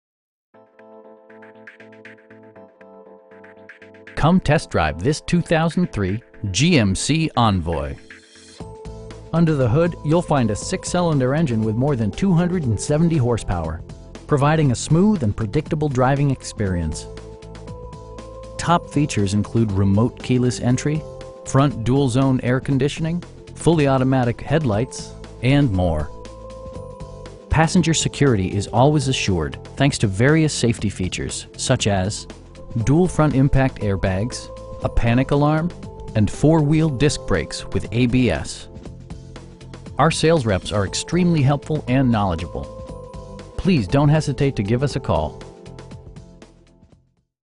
2003 GMC Envoy in Jackson, MS 39211
Gray-Daniels Chevrolet
6060 I-55 North

Come test drive this 2003 GMC Envoy. Under the hood you'll find a 6 cylinder engine with more than 270 horsepower, providing a smooth and predictable driving experience. Top features include remote keyless entry, front bucket seats, front dual-zone air conditioning, fully automatic headlights, tilt steering wheel, power door mirrors, cruise control, and more. Passenger security is always assured thanks to various safety features, such as: dual front impact airbags, a panic alarm, and 4 wheel disc brakes with ABS. Our sales reps are extremely helpful & knowledgeable. We'd be happy to answer any questions that you may have. Please don't hesitate to give us a call.